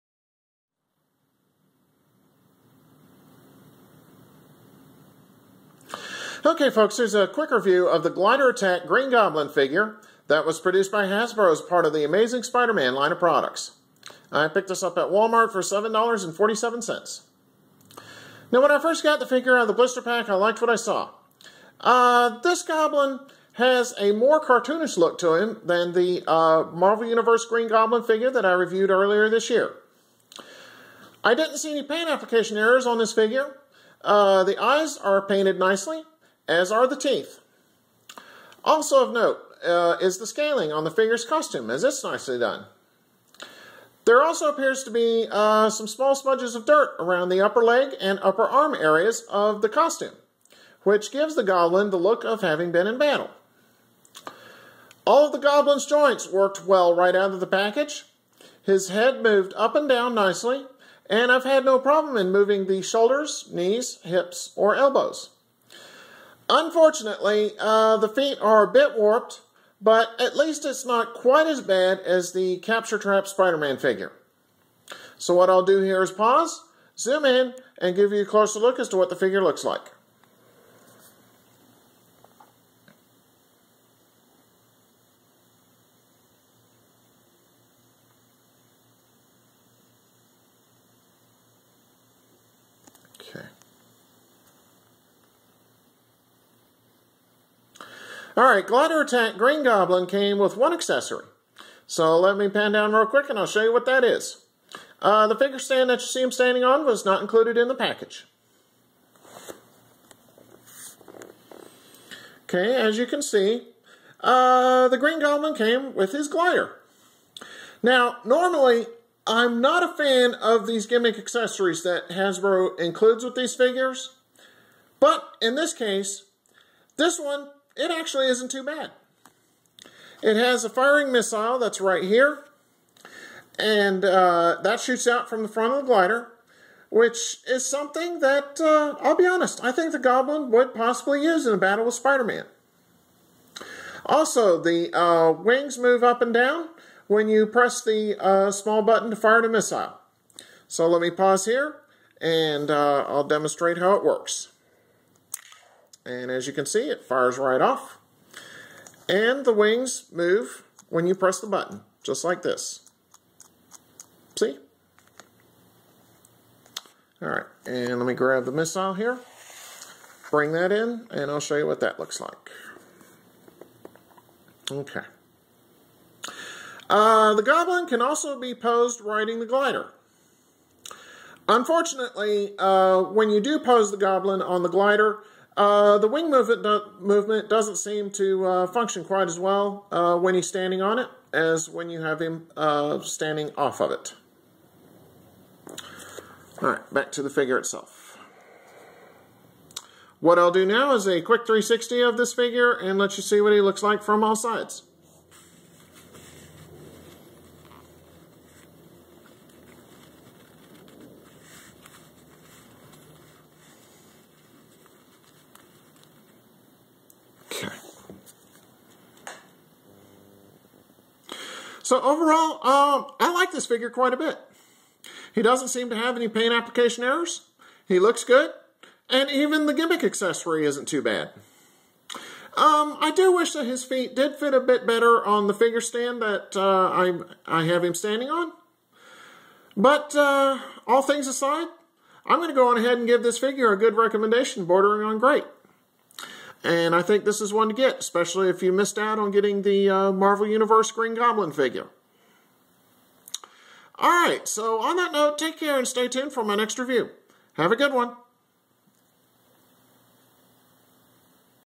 The Amazing Spider Man Glider Attack Green Goblin 1080p HD Figure Review
A review of the Glider Attack Green Goblin figure that was produced by Hasbro as a part of the Amazing Spider Man line of products.  This is part of the Comic Series.